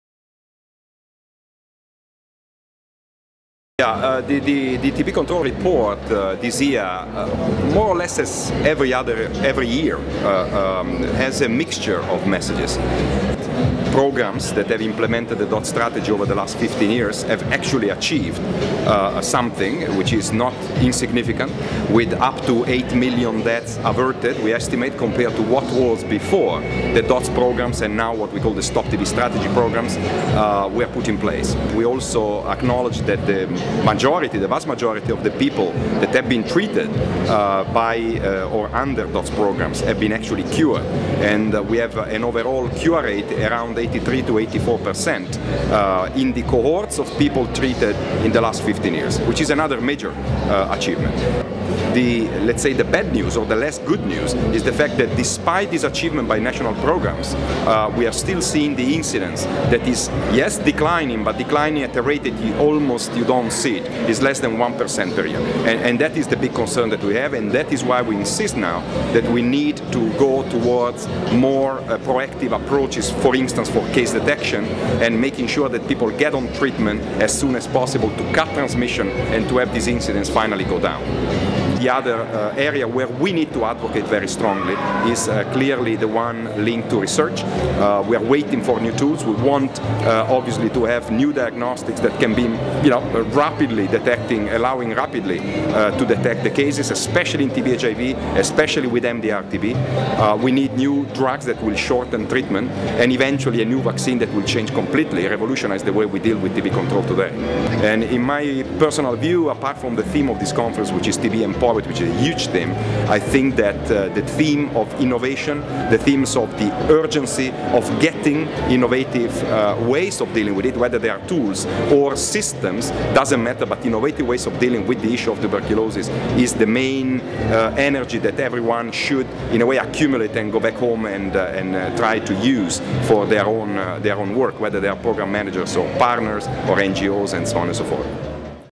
Union World Conference 2009: Mario Raviglione
At the Stop TB Symposium a day before the opening of the conference, Dr. Mario Raviglione and the WHO announced the latest statistics on TB from the Global TB Control Report. In this video Dr. Raviglione talks about the latest trends in TB control and the steps that are needed to reach the global targets for 2015.